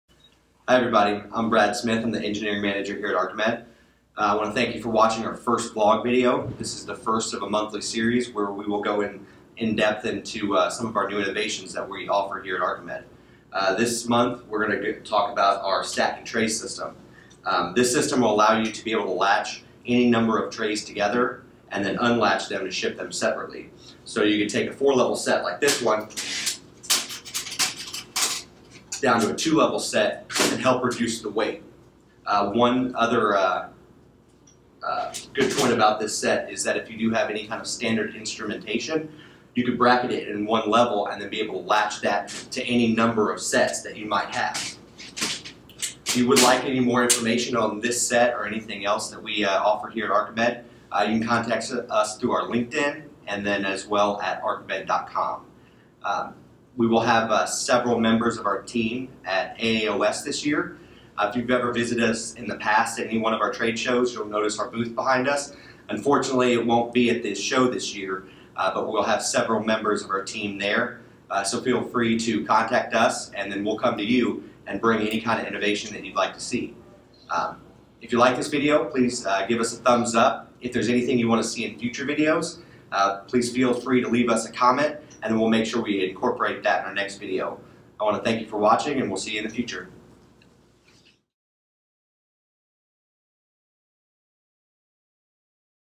Stacking Tray System
This is the first video of our new monthly vlog series.  Meet Brad Smith, our Engineering Manager, and learn about one of Arcamed's new products.